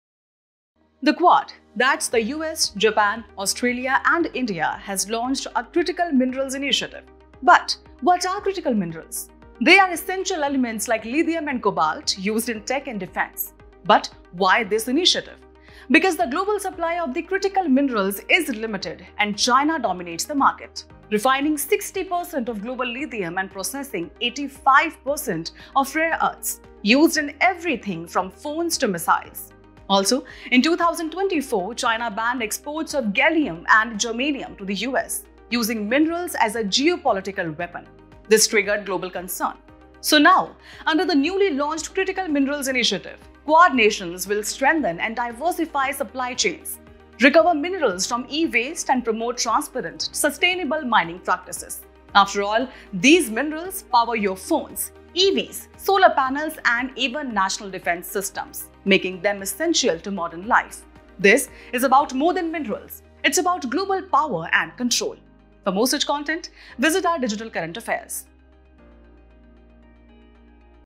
Daily Edge by Vision IAS | Quad Launches Critical Minerals Initiative
The Quad nations — US, Japan, Australia, and India — have launched a Critical Minerals Initiative to reduce dependence on China, which dominates global mineral processing.

With essential minerals like lithium and rare earths powering everything from phones to defence systems, the initiative aims to diversify supply chains, promote sustainable mining, and recover minerals from e-waste.

It’s not just about resources — it’s about securing global tech and strategic power.